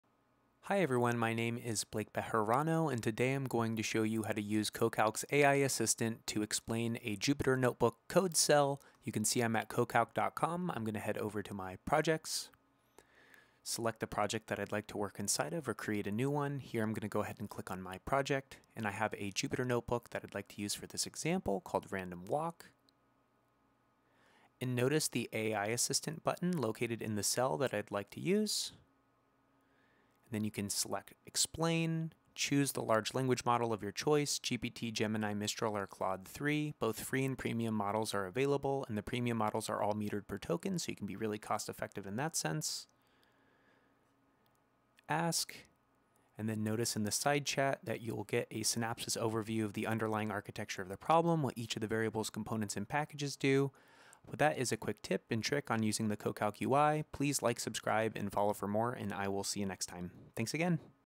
How to Use AI to Explain Jupyter Notebook Cells in CoCalc
Discover how to leverage AI to explain your Jupyter Notebook cells within CoCalc. This short, engaging tutorial will guide you through the process, helping you understand code better and faster.

By the end of this video, you'll know how to use the AI Assistant to get detailed explanations of your code, from underlying architecture to individual variables and components. This will greatly enhance your programming workflow, making coding more intuitive and insightful.

Whether you're a student, academic, or researcher, CoCalc's AI tools will boost your productivity and deepen your understanding of complex code. Join us and add a new dimension to your data analysis skills! For comprehensive guidance, make sure to visit the CoCalc documentation.